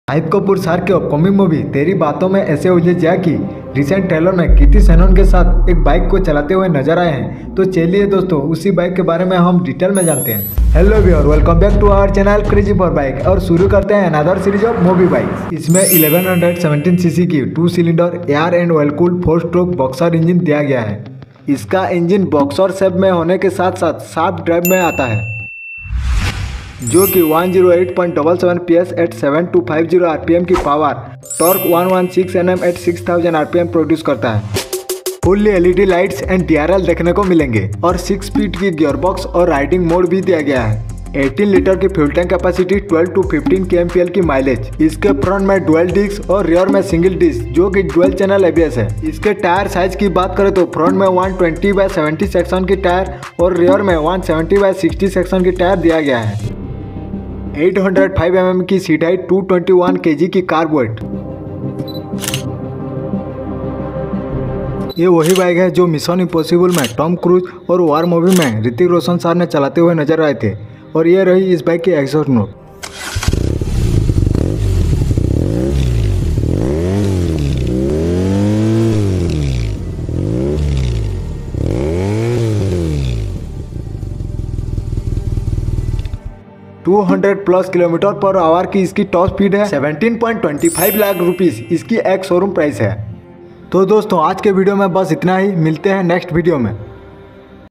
Movie Bike/Model/Specs/Top Speed/Price/Mileage
Teri Baaton Mein Aisa Uljha Jiya/Movie Bike/Model/Specs/Top Speed/Mileage, @CrazyForbike3

👉 Aaj ke Iss video mein hum Teri Baaton Mein Aisa Uljha Jiya movie Mmein jo bike dikhaya gaya hai usike bare mein hai,

👉इस चैनल के माध्यम से आप को Bikes से Related वीडियो मिलते रहेंगे